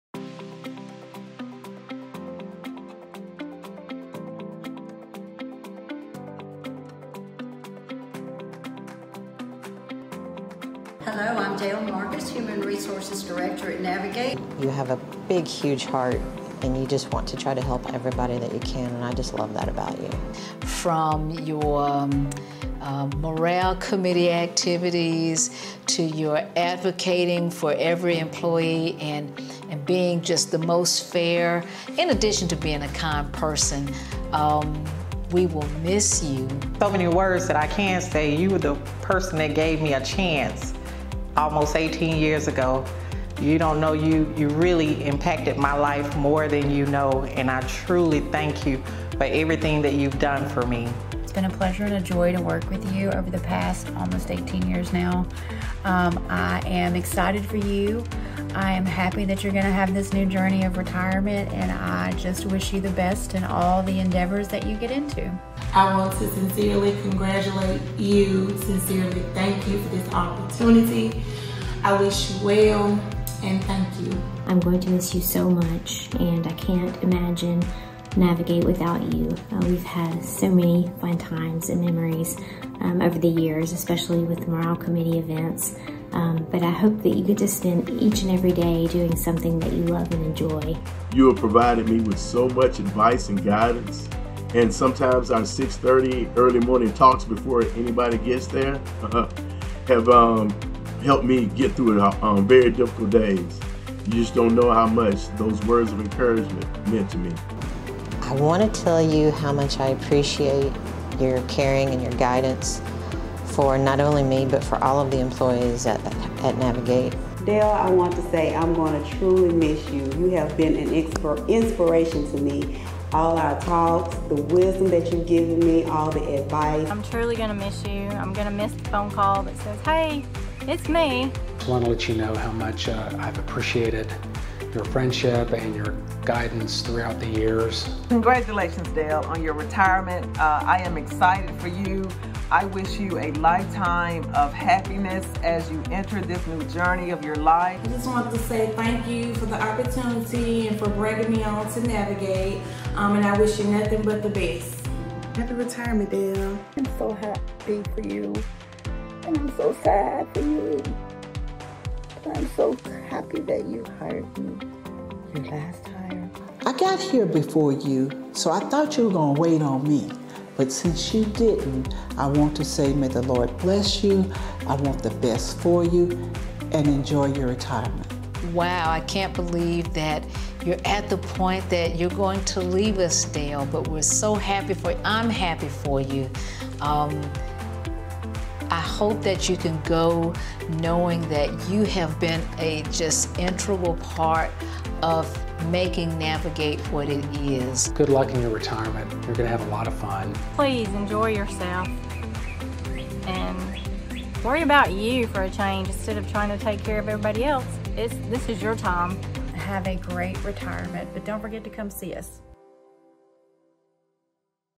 See You Soon, Dale!
Join the Navigate staff in wishing our longtime HR Director, Dale, a happy retirement.